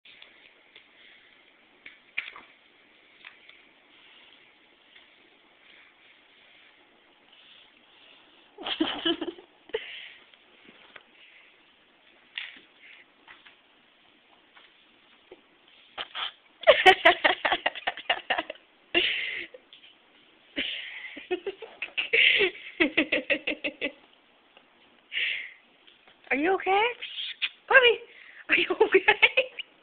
Crazy puppy eats his collar
he doesnt like his collar very much